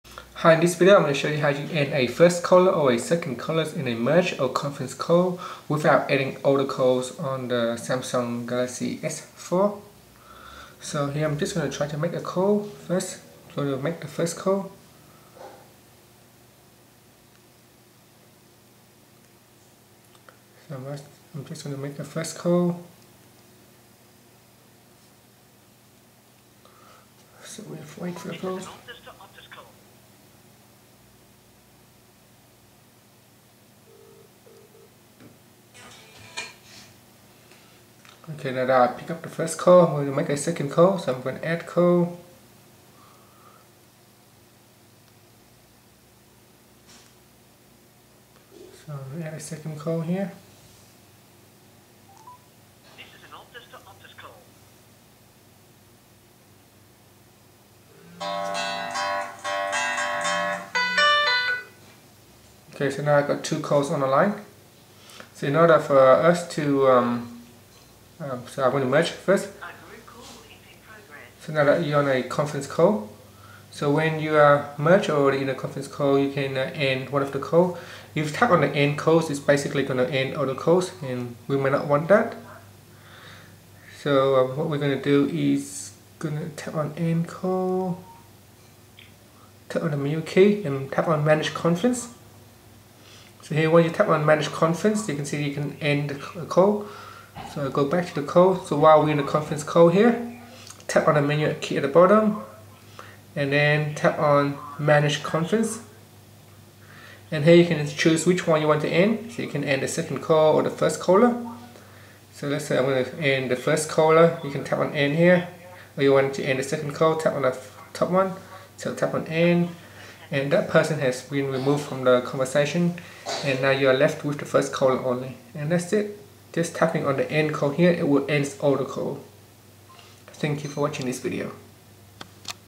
Samsung Galaxy S4: How to End Seperate Caller from a Merge / Conference Call
Learn how you can end just a caller form a merge or conference call on the Samsung Galaxy S4.

If you are on a conference call with 4 other people, you might just want to end one caller and have the others stay in the conference, in this case don't tap on the End button, other you will hang up everyone. See the video for step by step instruction.